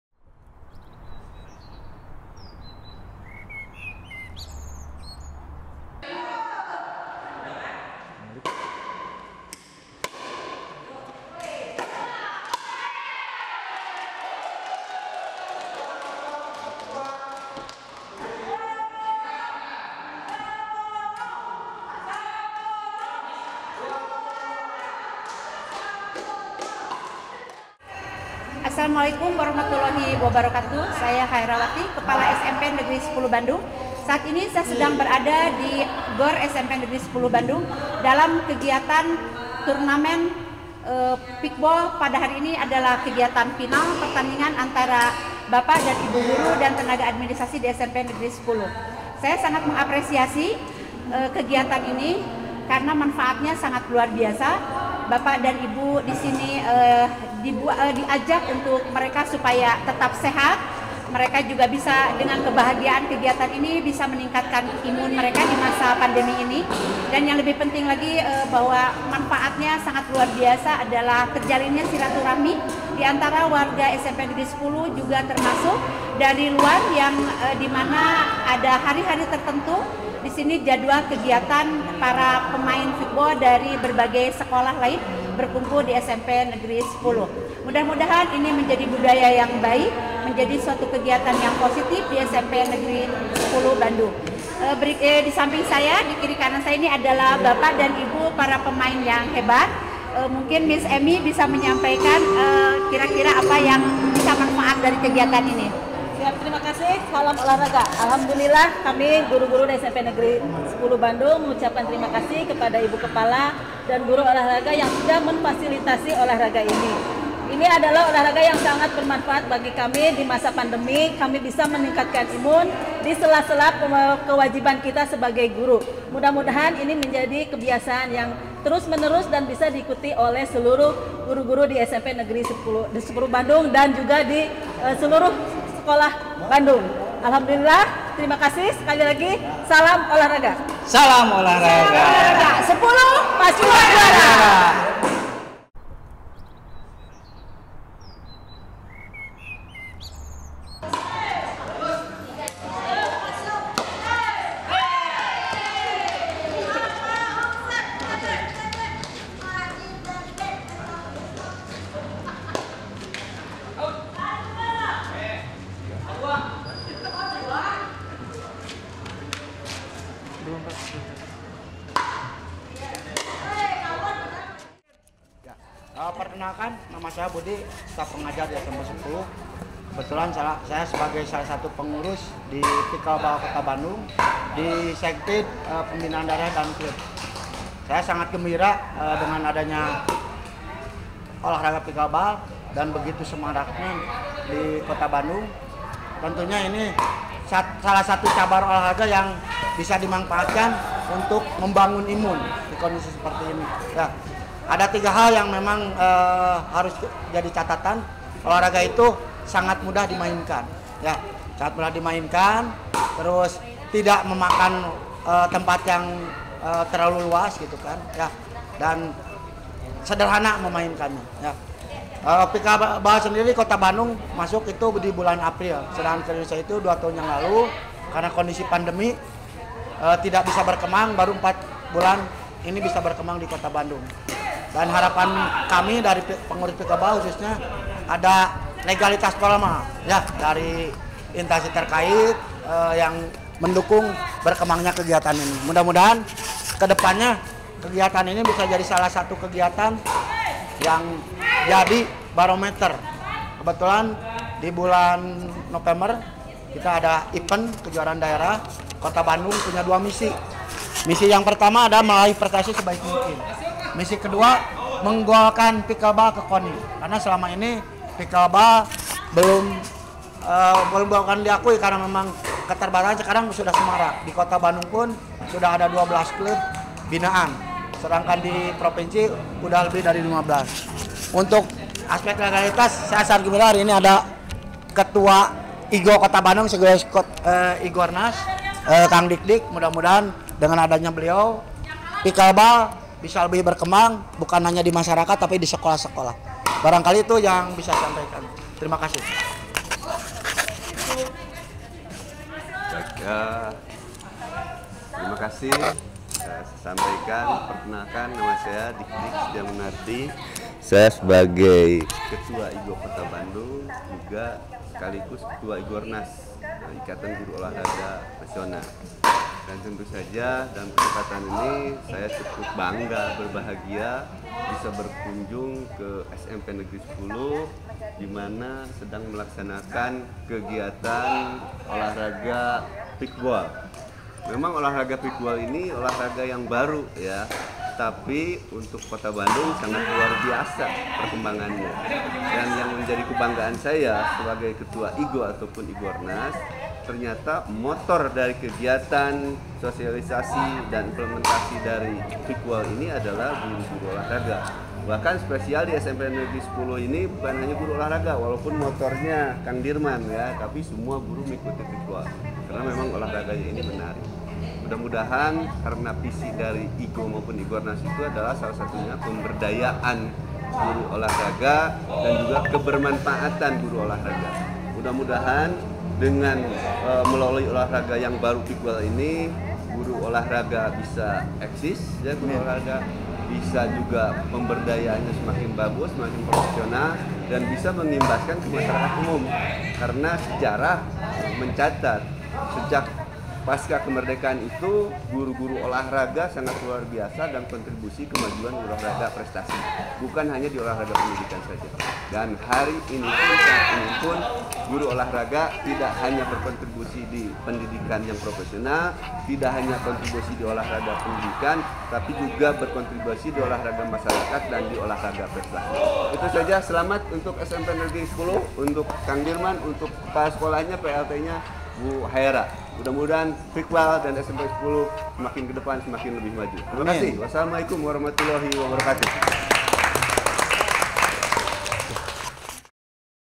Pickleball SMPN 10 Kota Bandung, Meningkatkan Konsistensi Sosialisasi Olahraga Prestasi
Bandung, Prabunews.com - Kegiatan olah raga Pickleball sebagai sebuah cabang olahraga baru sepertinya sangat digemari dan diterima masyarakat secara antusias.

Hal ini terlihat pada civitas akademika SMPN 10 Kota Bandung.

Pada hari ini kegiatan olah raga yang memadukan kecakapan bulutangkis, tenis lapangan dan tenis meja ini terlihat di sekolah tersebut.Sebuah turnament sedang berlangsung dan sangat meriah.

Khaerawati, S.Pd, M.M.Pd Plt. Kepala SMPN 10 Kota Bandung mengatakan bahwa Kegiatan Pickleball di sekolah yang dipimpinnya sangat rutin dilakukan dan gelora para pesertanya sangat semangat.

Saya sangat mendukung sekali kegiatan olah raga ini, disamping kami memiliki fasilitas lapangan indoor dan outdoor, kegiatan olah raga ini pun memiliki nilai-nilai baik dalam membangun kebugaran warga sekolah yang bermuara pada bertambahnya imunitas serta dengan kegiatan olah raga nilai-nilai kebersamaan dan persaudaraan pun dapat tumbuh dengan baik.

Khaerawati, S.Pd, M.M.Pd Plt. Kepala SMPN 10 Kota Bandung Bersama Dikdik Setia Munardi ,S.Pd, M.MPd Ketua Ikatan Guru Olah Raga (IGO) Nasional

Dirman Budiman S.Pd Guru olah raga sekaligus pengurus Pencab Pickleball Kota Bandung mengatakan bahwa dirinya sangat berharap agar olah raga ini dapat disosialisasikan di berbagai kalangan baik di lingkungan pendidikan maupun di kalangan masyarakat mengingat fasilitas yang diperlukannya pun cukup sederhana.

Pada kesempatan event lomba ini dihadiri oleh Dikdik Setia Munardi ,S.Pd, M.MPd Ketua Ikatan Guru Olah Raga (IGO) Nasional.

Dalam sambutannya Dikdik Setia Munardi ,S.Pd, M.MPd mengatakan sangat mengapresiasi geliat olah raga Pickleball di SMPN 10 Kota Bandung.

Beliau berharap agar kegiatan olah raga Pickleball ini bisa terus konsisten tentunya dengan terus mengaktifkan berbagai event yang terkait didalamnya.

(Kang Amat)